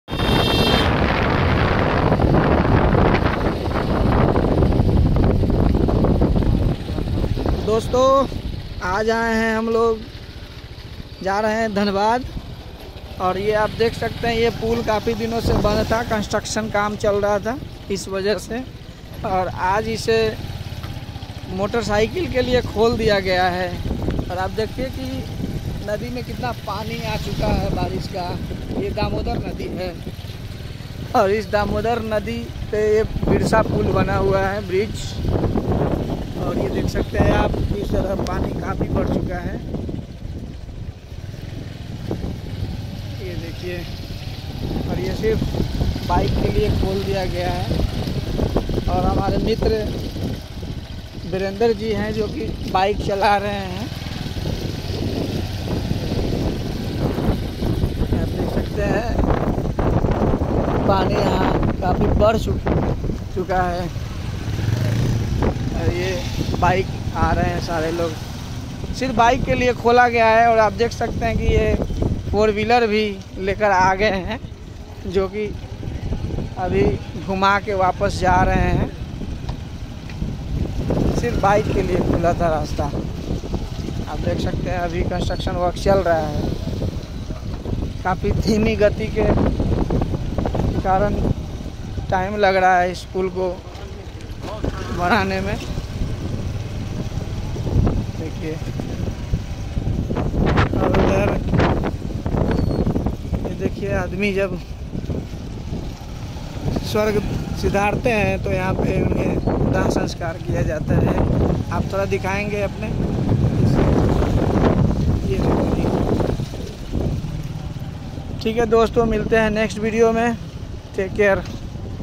बोकारो से धनबाद का सफर।
बोकारो को धनबाद से जोड़ने वाला बिरसा पुल आज से  दो पहिया वाहन  के लिए चालू